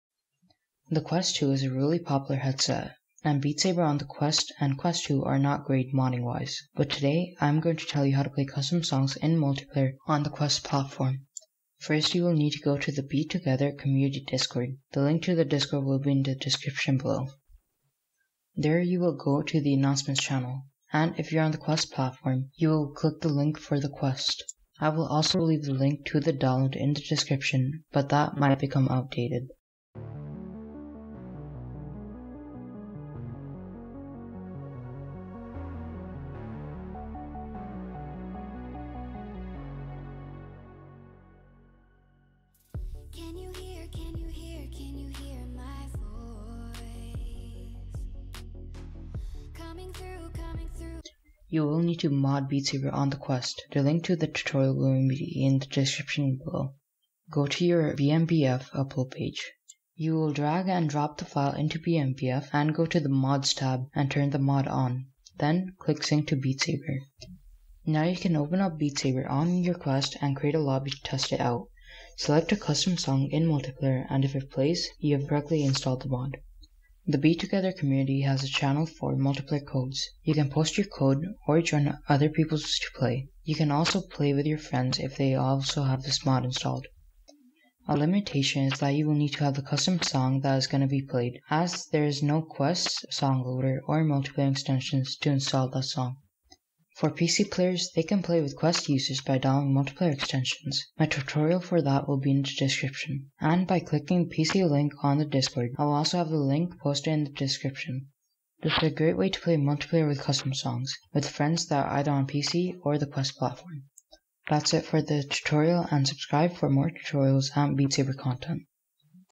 Oculus Quest 2 Custom Songs Multiplayer
Note: The Mod is in an early stage and it may have bugs. Quick play currently is not available yet, and I have removed the direct download links since I can't keep them updated
This to my knowledge is the first tutorial on how to play custom songs in multiple on the Oculus Quest and Quest 2. Note: For BMBF don't install version 1.13.2, install 1.13.1 or 1.13.0
Links: